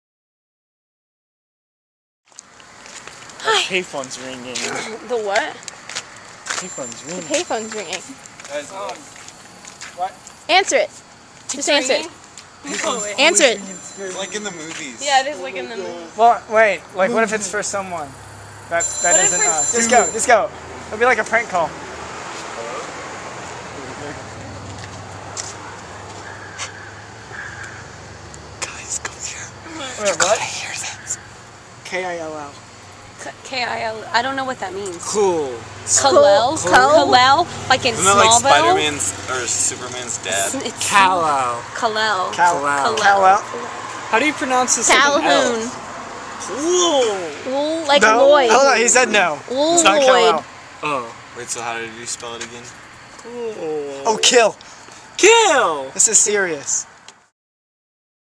Payphone Call
Do you answer a payphone when you don't know who's on the other end of the line?